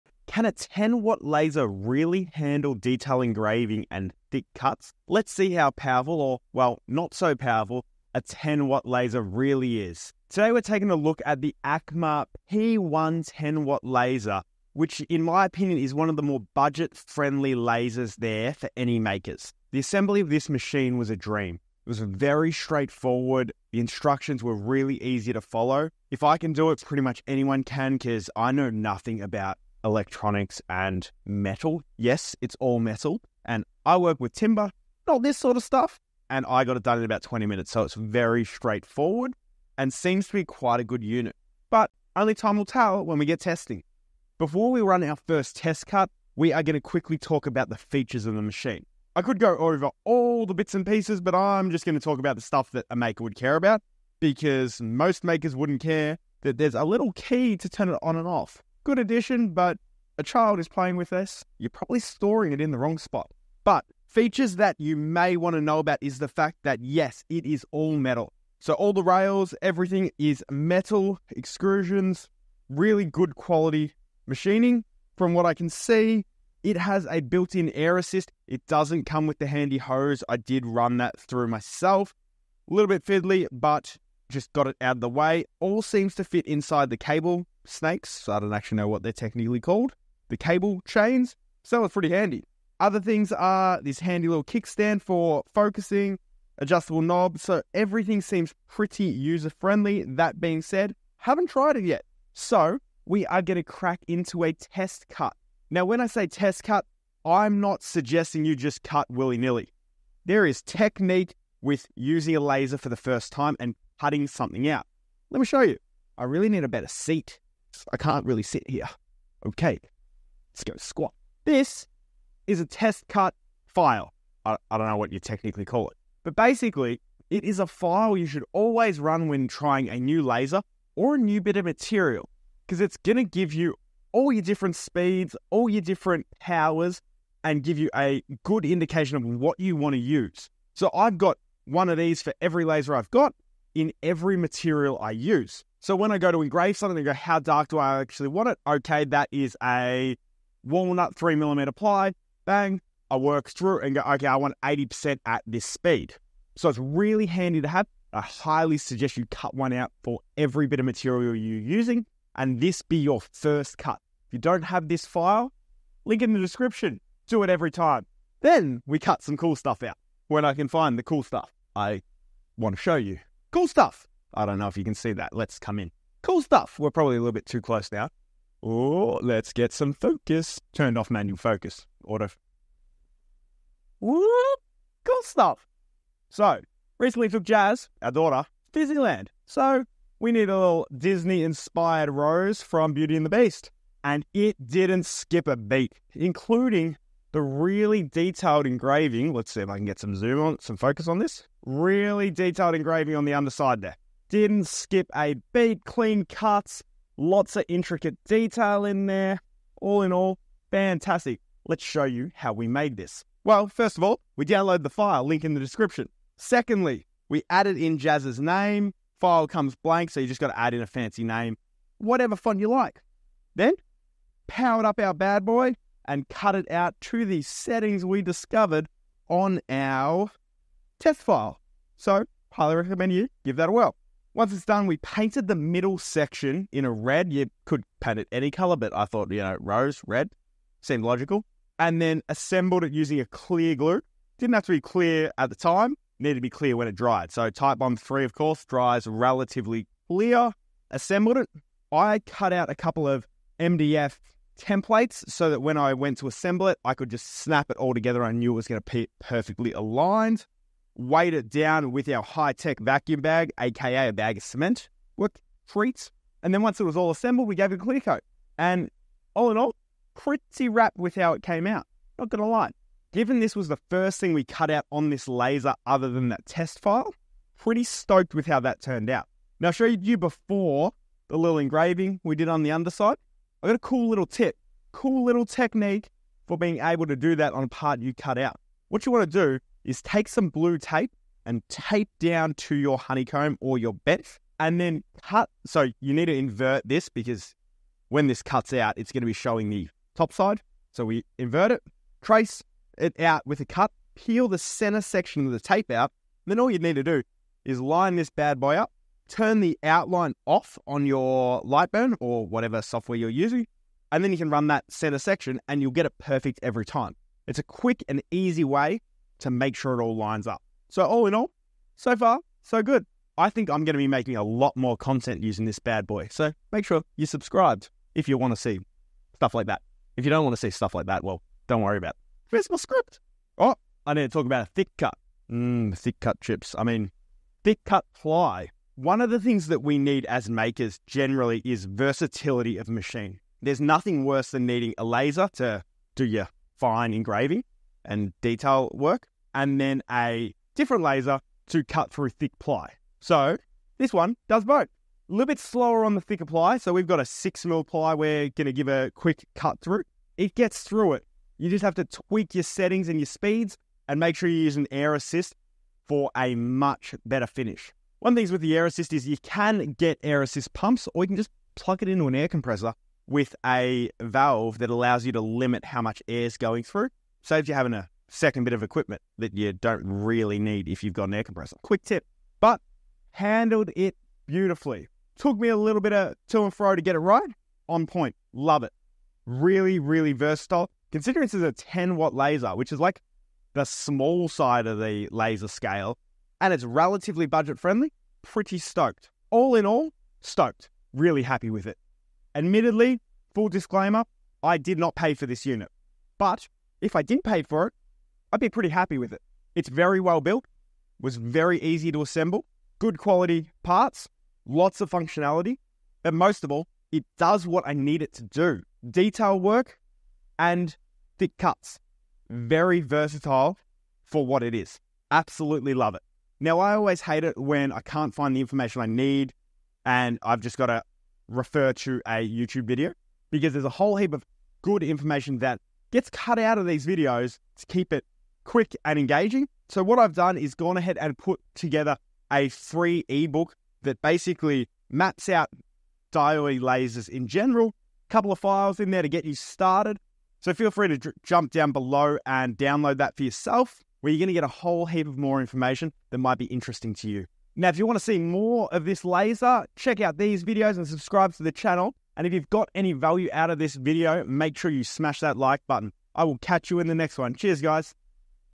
Surprising Results From a 10W Laser Engraver!
Discover the ACMER P1 10W Laser Engraver, one of the best affordable lasers for makers looking to balance budget and performance! In this video, we put the ACMER P1 to the test, covering everything from detailed engraving to cutting thick materials. If you're a maker seeking versatility, this laser could be the perfect addition to your workshop.

In this review, we'll show you:
      • The ease of assembly (even for non-tech-savvy users)
      • A detailed test cut using different settings
      • Tips for getting the most out of your laser, including intricate engraving techniques and thicker material cuts
      • Why this 10W laser punches above its weight in versatility

Whether you're a beginner or an experienced maker, this review will help you determine if the ACMER P1 is right for your next project. Don't forget to download the test cut file in the video description and give it a try with your own laser!

00:00 – Can a Budget Laser Actually Handle This?
Set up the challenge: 10W laser, detail engraving vs thick cuts

00:20 – First Impressions & Assembly - Ease of setup, build quality, and who it's for.
00:45 – Features That Matter to Makers - Metal frame, air assist, cable chains, and other useful features.
01:42 – First Test Cut: Why You Should Always Do This - Why test grids matter, and how to use them properly.
02:52 – Detail Engraving Test: Beauty & the Beast Rose - Showcase the engraving quality on 3mm ply — including tips.
03:32 – Step-by-Step: How We Made It - File setup, adding text, painting, glue-up, alignment tricks.
04:39 – Bonus Tip: Perfect Underside Engraving - Blue tape method for flipped engravings — quick hack.
05:33 – Can It Cut Thick Material? (6mm Ply Test) - Thick cut test and air assist advice.
06:38 – Final Verdict: Versatility & Value - What it can (and can’t) do well for a budget laser.
07:06 – Free Laser Cutting eBook + File Bundle - Mention your free resources and where to get them.
07:37 – What’s Next? - Subscribe prompt, what content is coming, CTA to like the video.

🔔Unlock Your Creativity with KJH Woodworking: Where Projects Come to Life and Skills Take Flight.